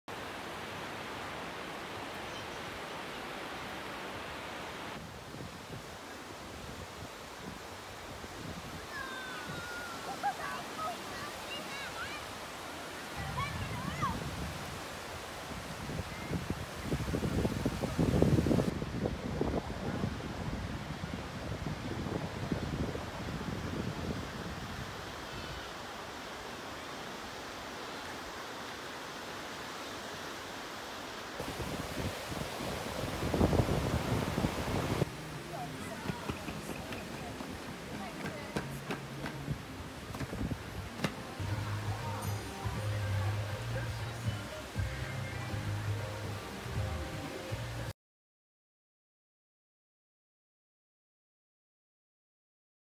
Sunset at White Sand Beach on Koh Chang
Enjoying the sunset at Koh Chang's most popular beach: White Sand Beach (Hat Sai Khao).